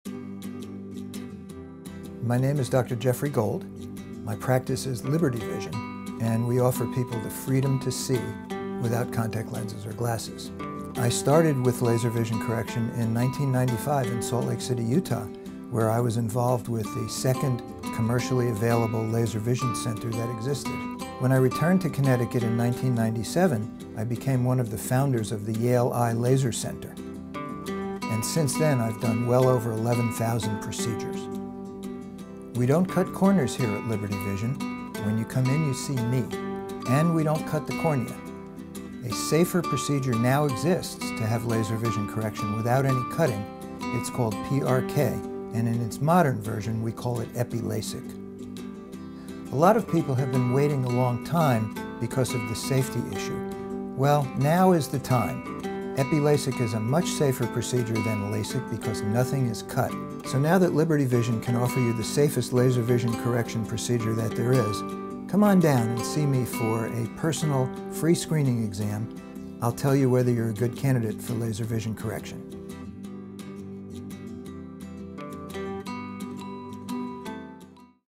ESC - Liberty Vision Yelp Video
This is a video Escape The Frame Media was hired to produce for The Liberty Vision through Yelp.